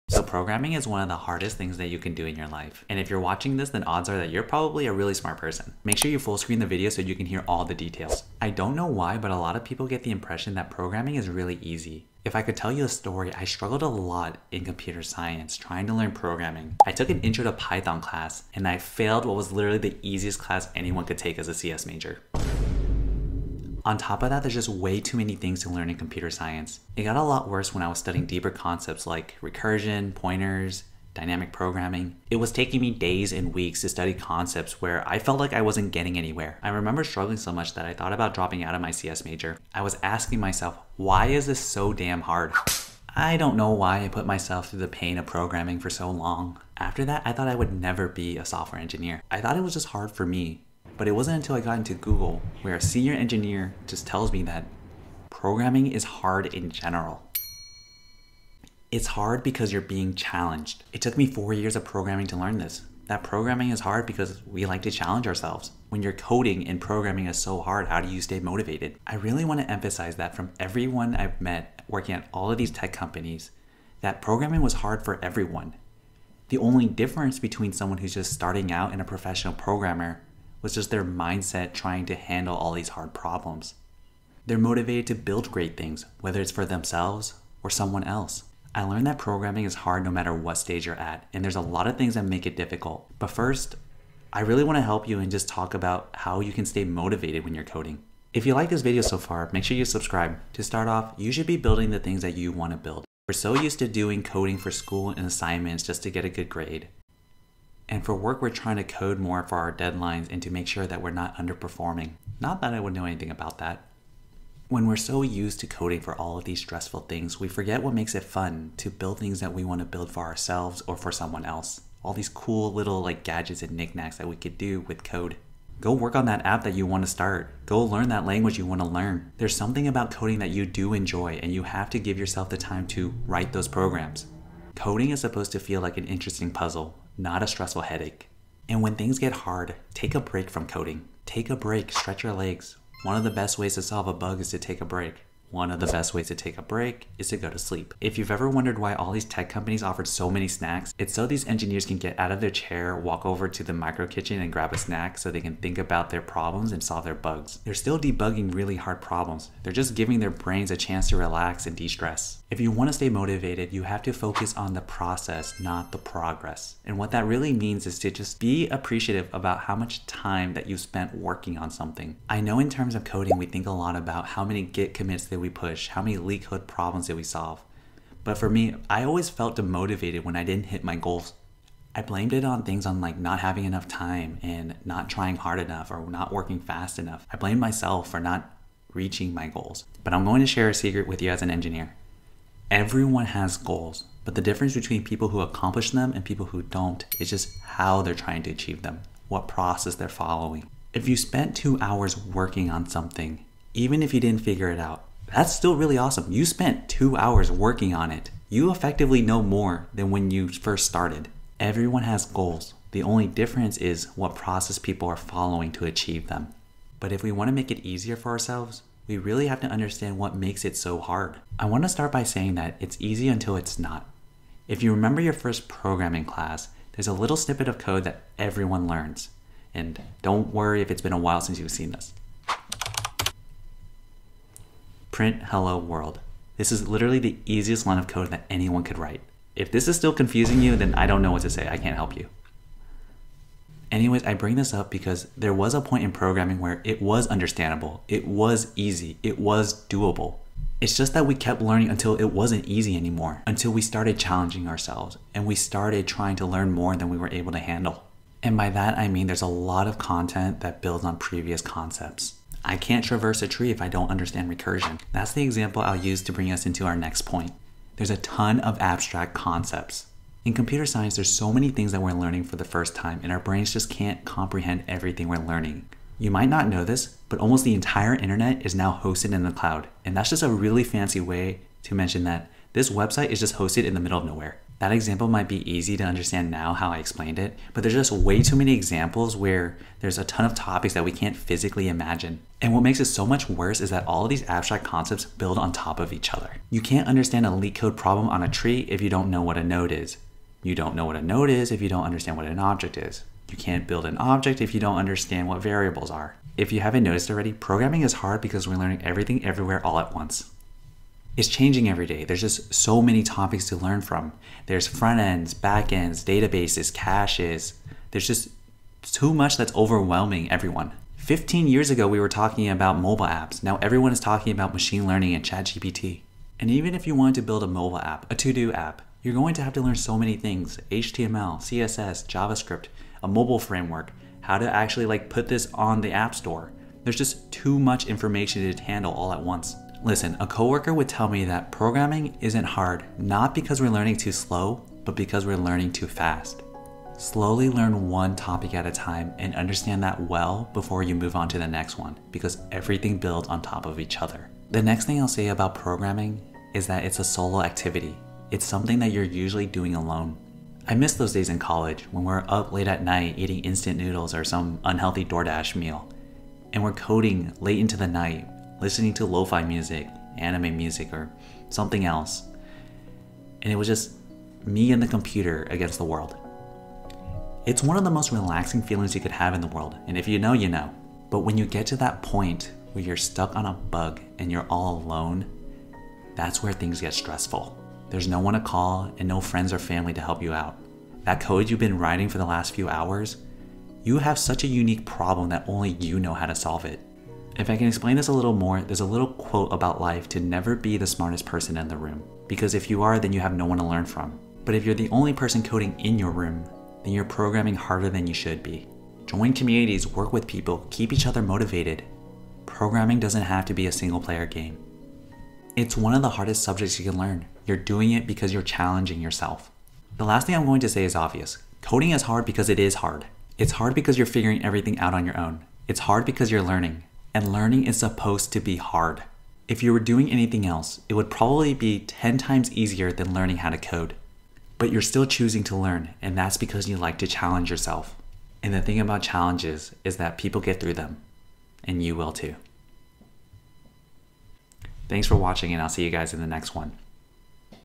This Is Why Programming Is Hard For You.
Programming is hard for everyone because programming is hard. It doesn't matter if you went to an ivy league or got a CS degree, programming is a difficult subject.

Even though it's hard for everyone, the most successful programmers are the ones who are motivated even if it's hard.